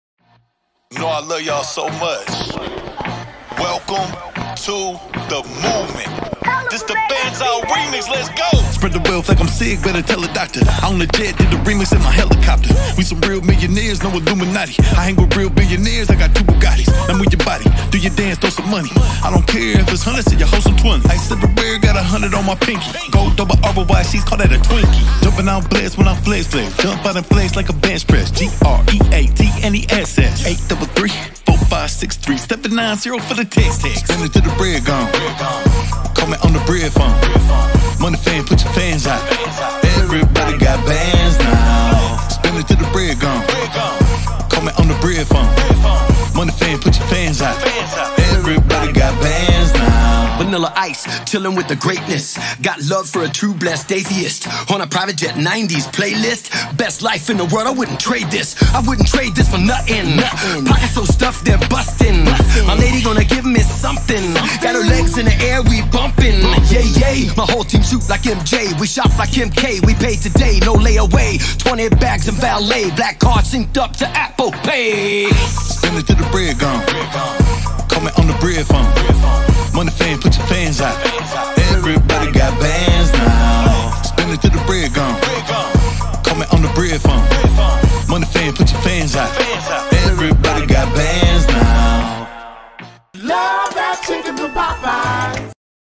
CHICKEN SANDWICH DELUXE BACON, Popeyes France (2023)🐔🐷
Généreux filet de poulet mariné pendant 12 heures dans notre mélange d’épices cajun, pané à la main et frit sur place. Servi avec deux tranches de bacon, une tranche de cantal, de la sucrine croquante, de la mayonnaise et un bun brioché toasté ultra moelleux.